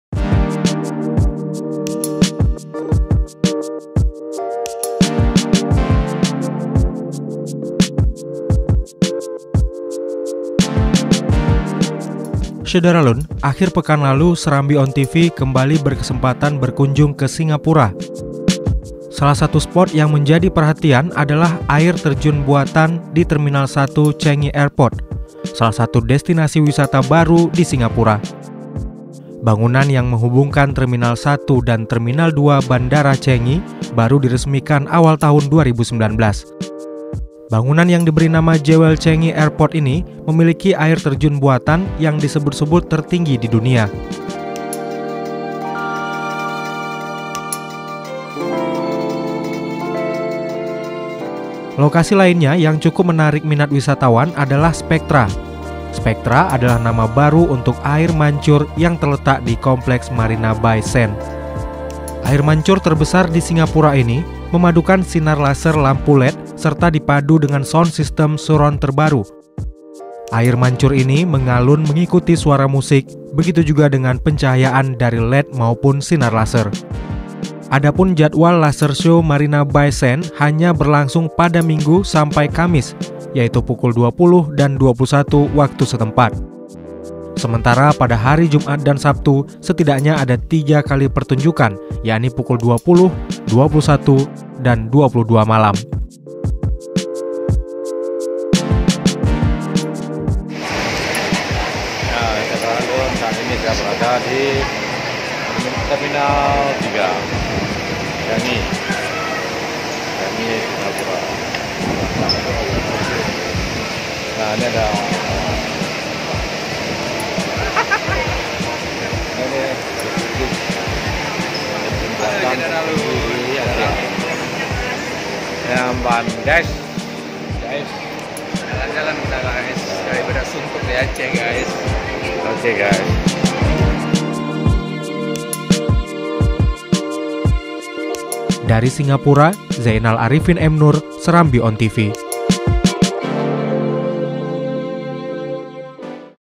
Jalan-jalan ke Singapura, Melihat Spectra dan Air Terjun Buatan
SERAMBITV.COM, SINGAPURA – Akhir pekan lalu, serambiontv kembali berkesempatan berkunjung ke Singapura.

Salah satu spot yang menjadi perhatian adalah air terjun buatan di Terminal 1 Changi Airport, menjadi destinasi wisata baru di Singapura.

Bangunan yang menghubungkan Terminal 1 dan Terminal 2 Bandara Changi baru diresmikan awal tahun 2019.

Bangunan yang diberi nama Jewel Changi Airport ini memiliki air terjun buatan yang disebut-sebut tertinggi di dunia.

Air terjun ini berada di tengah-tengah hutan tiruan yang berisi lebih dari 2.000 pepohonan dengan 120 spesies tanaman dari berbagai negara.

Bangunan Jewel ini dibangun khusus sebagai atraksi wisata, pusat perbelanjaan, akomodasi, dan restoran bagi wisatawan lokal serta luar negeri.

Lokasi lainnya yang cukup menarik minat wisatawan adalah Spectra.

Spectra adalah nama baru untuk air mancur yang terletak di Kompleks Marina Bay Sands.

Air mancur terbesar di Singapura ini memadukan sinar laser, lampu LED, serta dipadu dengan Sound system surround terbaru.

Air mancur ini mengalun mengikuti suara musik, begitu juga dengan pencahayaan dari LED maupun sinar laser.

Adapun jadwal laser show Marina Bay sands hanya berlangsung pada Minggu sampai Kamis, yaitu pukul 20.00 dan 21.00 waktu setempat.

Sementara pada hari Jumat dan Sabtu, setidaknya ada tiga kali pertunjukan yakni pukul 20.00, 21.00, dan 22.00 malam.